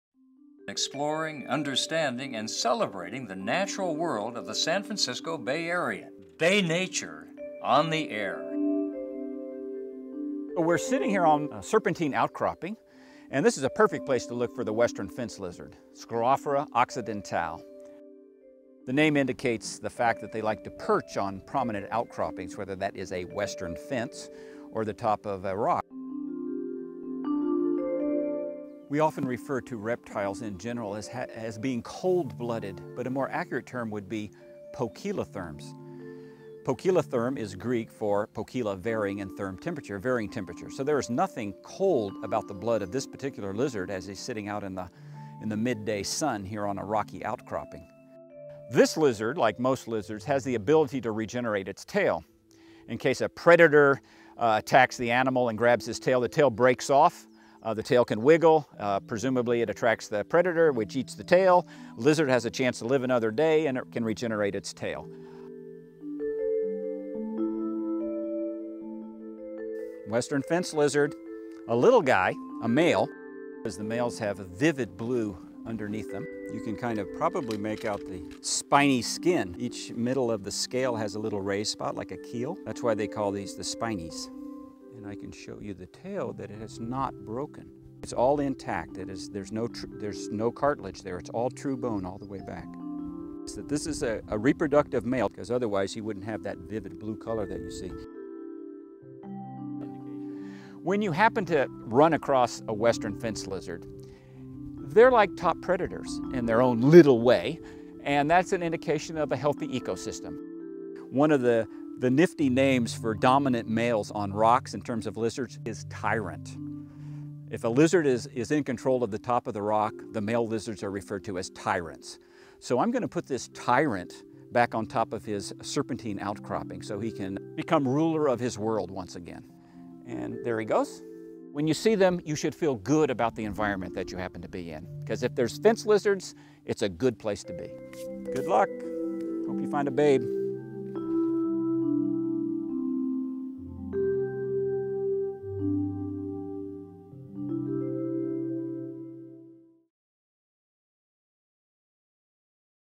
Western Fence Lizard
Western fence lizards are a common sight in Bay Area parks on sunny days. "When you see them," says naturalist Michael Ellis, "you should feel good, because if there's fence lizards, that's a good place to be."